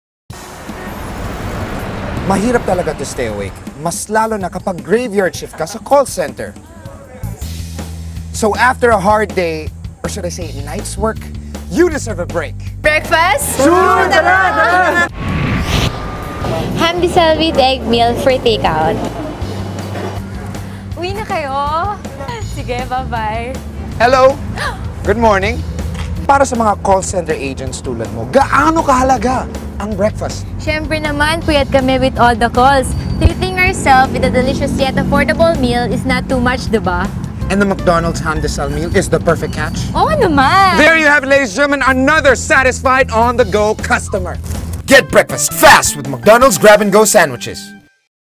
Mc Donald's grab and go TVC call center agent w/ Mark Abaya
TVC of Mc Donald's with Mark Abaya